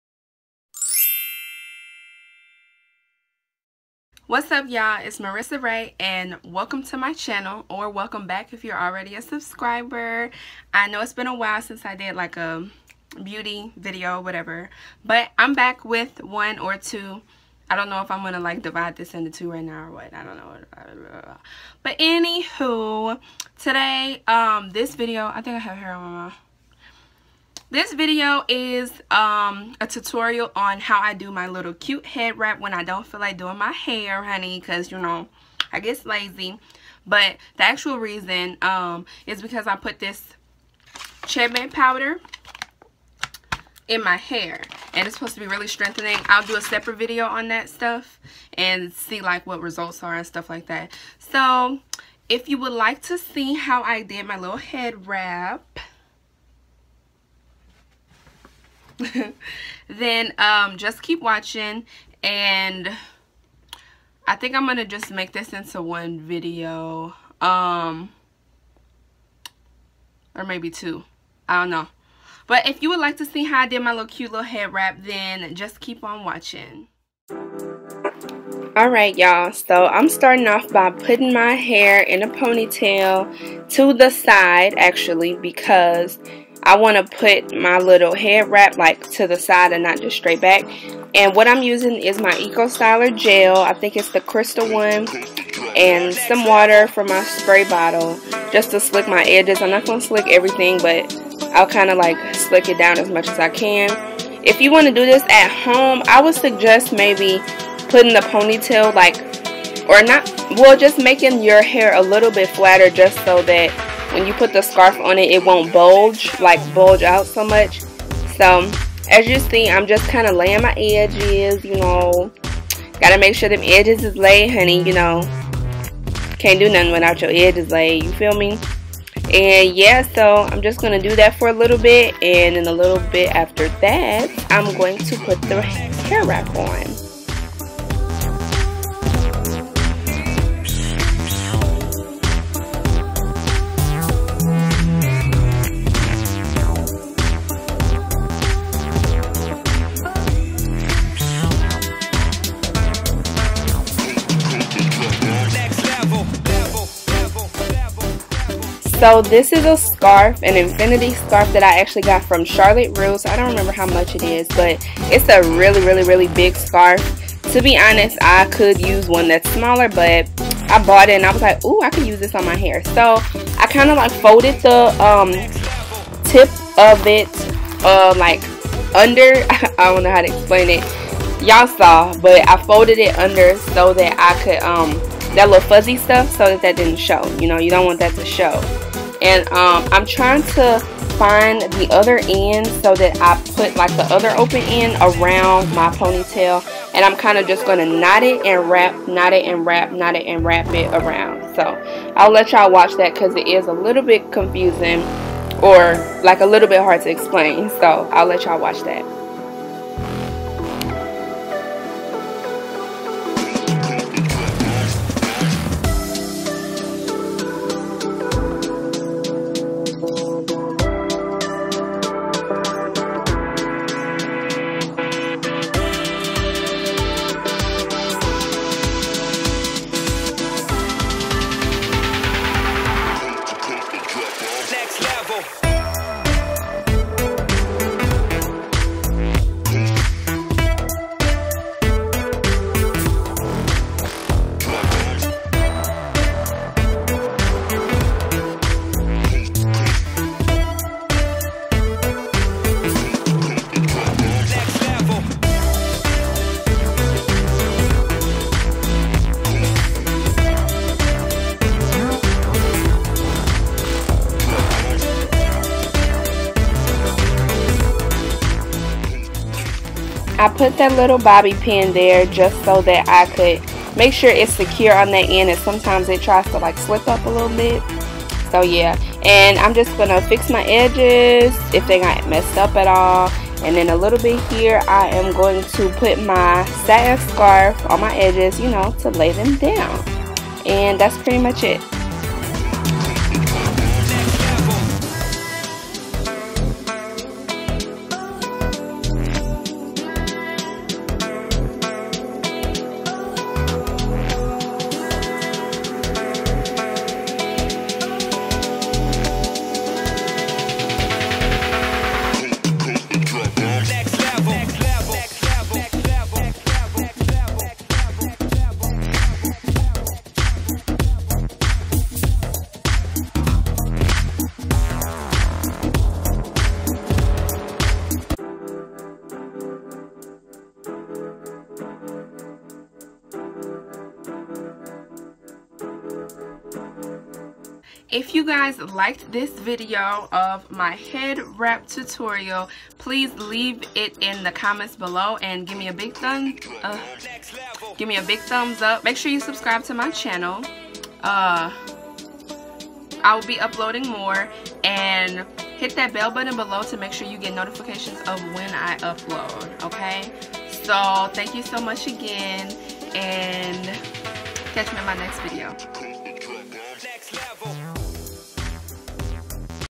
Head Wrap Tutorial
Hello everyone!!  Oh and make sure your notifications are on so you'll never miss a video :)

The only product I used in this video was water and Eco Styler Gel.

SC: maryysssaa